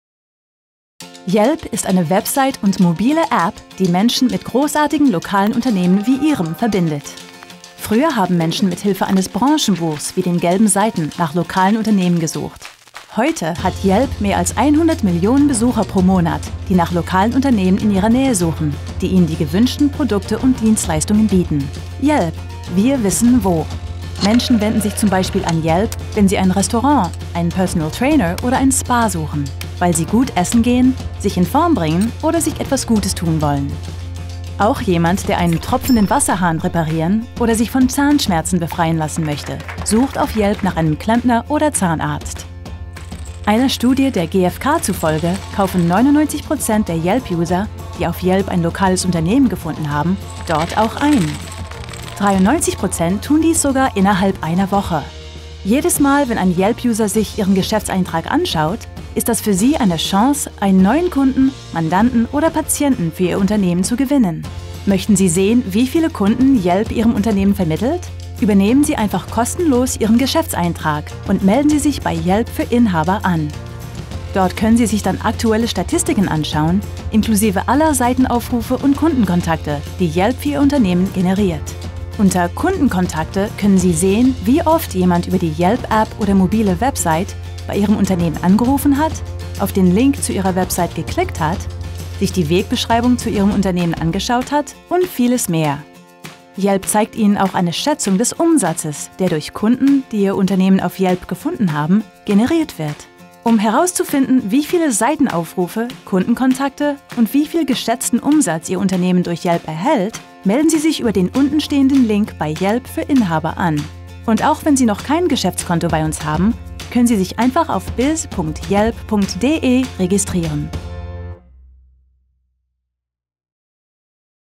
Einführung in Yelp für Geschäftsinhaber
Sehen Sie hier eine Einführung für Geschäftsinhaber auf Yelp. Dieses Video zeigt die Funktionen und Vorteile des kostenlosen Geschäftskontos auf Yelp für Inhaber.